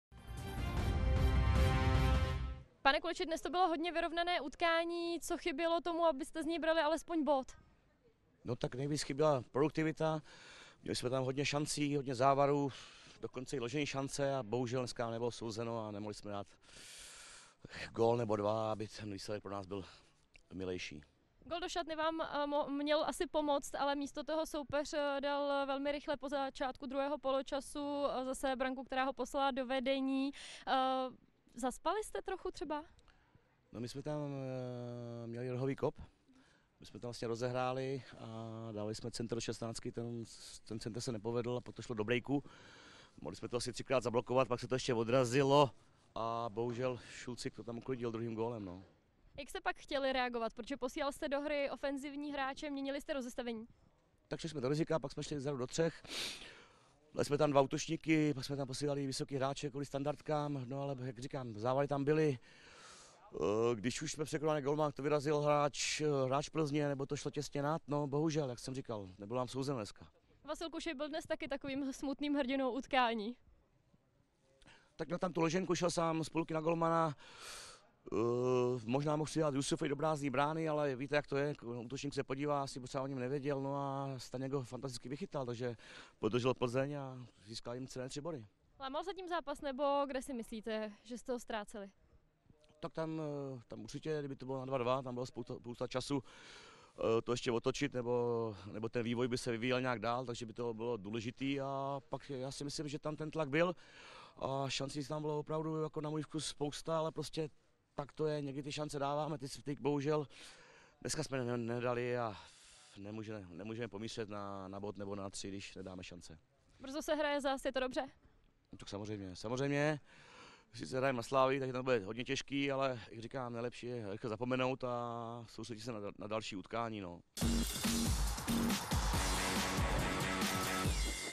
Kulič: Dnes nám bohužel nebylo souzeno, nejvíc chyběla produktivita
Mladá Boleslav neuspěla v boji o lepší umístění v tabulce, s Plzní prohrála 1:3. „Nejvíc chyběla produktivita, měli jsme tam hodně šancí, dokonce i vyložené šance. Bohužel nám nebylo dnes souzeno a nemohli jsme dát gól nebo dva, aby pro nás byl ten výsledek milejší,“ řekl zklamaný trenér Kulič.